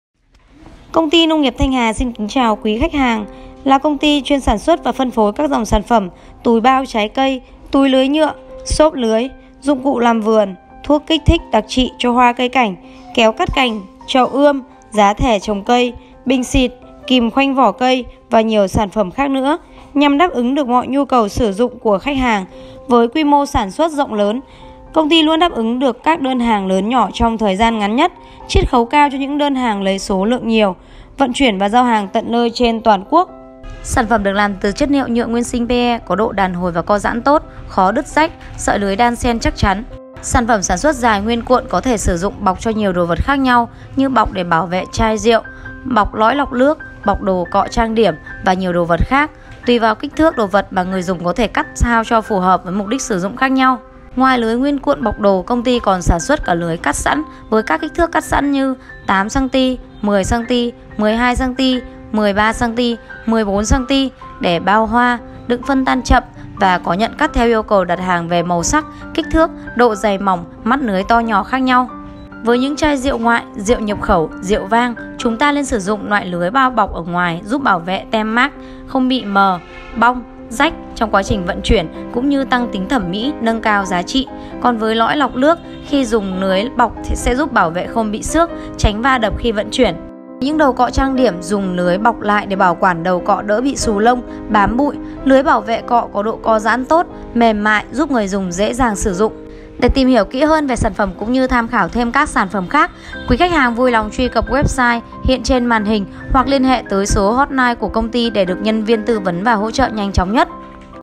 Bao bọc đồ bằng túi lưới nhựa/Nongnghiepthanhha.vn
Với những chai rượu ngoại, rượu nhập khẩu, rượu vang chúng ta nên sử dụng lưới nhựa bao bọc ở ngoài nhằm giúp bảo vệ tem nhãn mác k bị mờ, bong, rách trong quá trình vận chuyển cũng như tăng tính thẩm mỹ nâng cao giá trị. Còn với lõi lọc khi dùng túi lưới bọc sẽ bảo vệ không bị xước, tránh va đập khi vận chuyển.